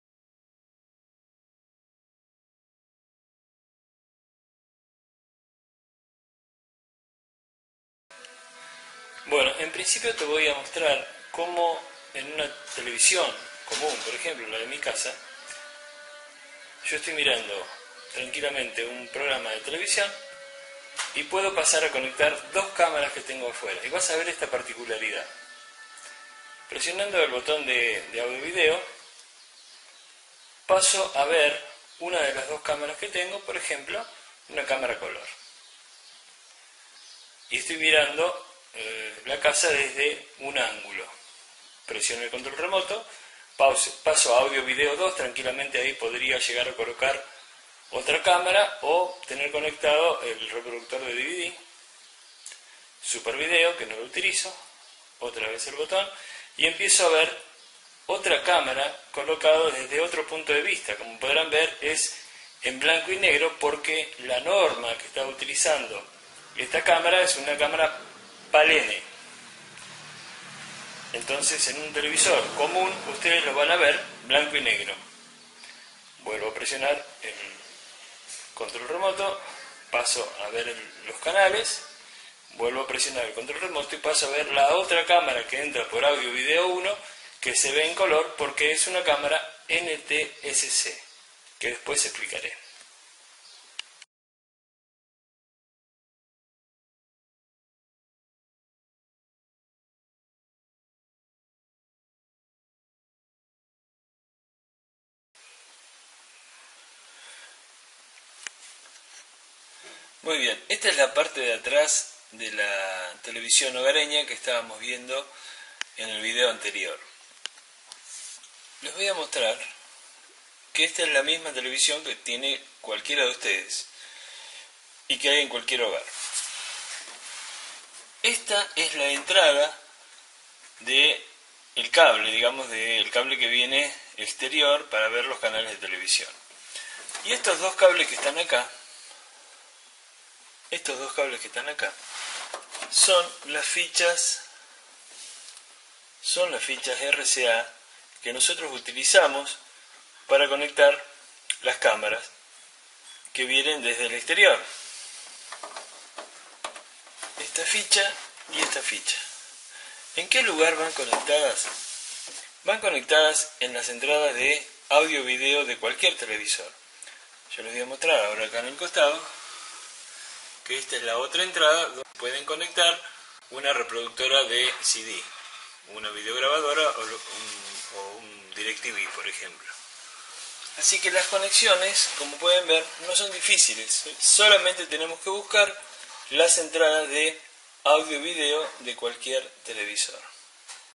video de eleccion de camaras con remoto y conexionado en televisor
Aqui te muestro como cambiamos desde el remoto de una TV hogareña para ver nuestras camaras exteriores y donde conectarlas en dicha TV